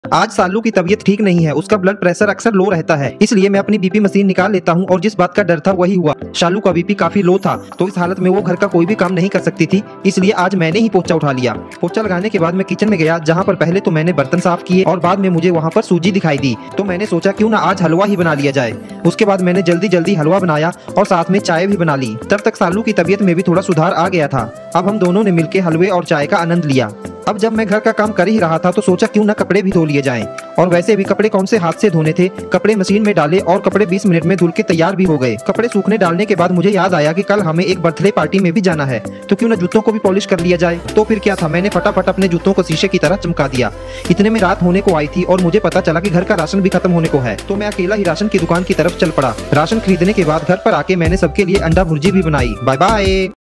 🥲Shalu Ki tabiyat hui Kharab🥹
Shalu Ki tabiyat hui kharaab

Aaj shalu ki tabiyat kharaab ho gyi thi isliye aaj ghar ke saare kaam mujhe hi krne pde.